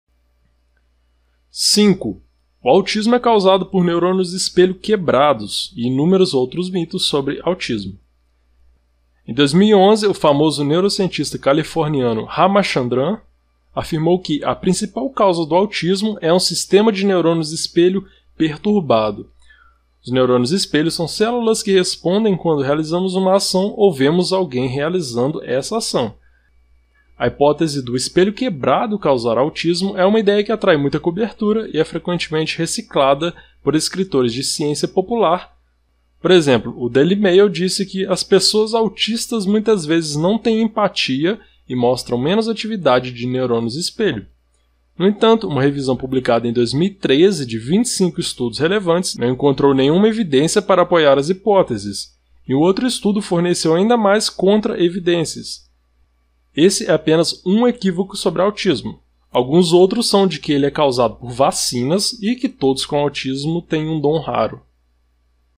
[MP 5/11] Mitos sobre Autismo: Neurônios espelho, Vacinas, Dom raro?
“A principal causa do autismo é um sistema de neurônios espelho perturbado”?

Esse vídeo faz parte da série Mitos da Psicologia (5/11).